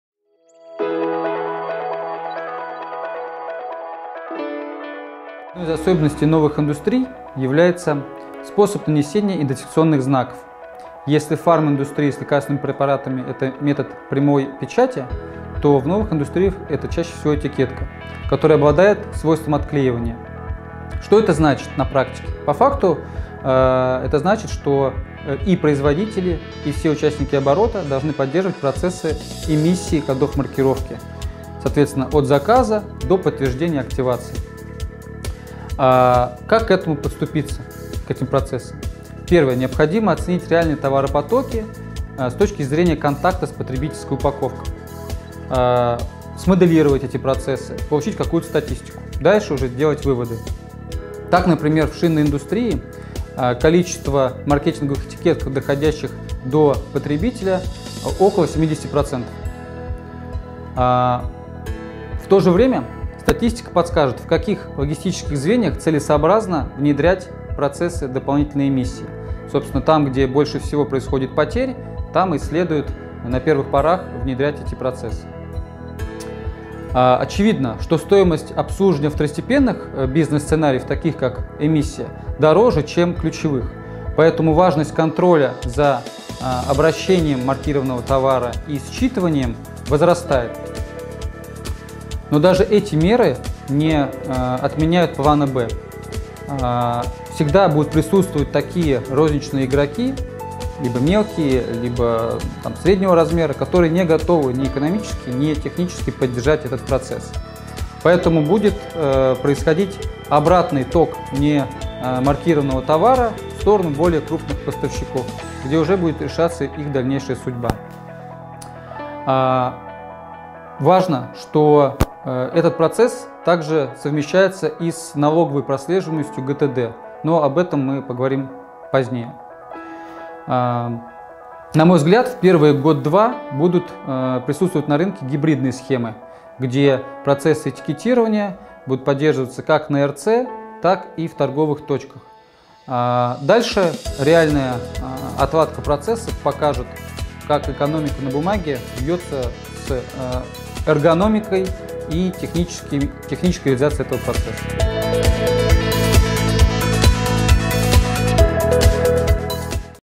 Ключевые особенности маркировки новых индустрий
Мы запустили серию экспертных видеороликов об актуальных вопросах из мира маркировки.
В первом выпуске Евгений Роднянский, операционный директор Utrace, рассказывает о ключевых особенностях в новых индустриях:
•        Что нужно знать о способе нанесения идентификационных знаков, чтобы избежать проблем?
•        Как подступиться к процессу эмиссии кодов маркировки?
•        В каких логистических звеньях целесообразно внедрять процессы дополнительной эмиссии?
•        Почему возрастает важность контроля за обращением и считыванием маркированного товара?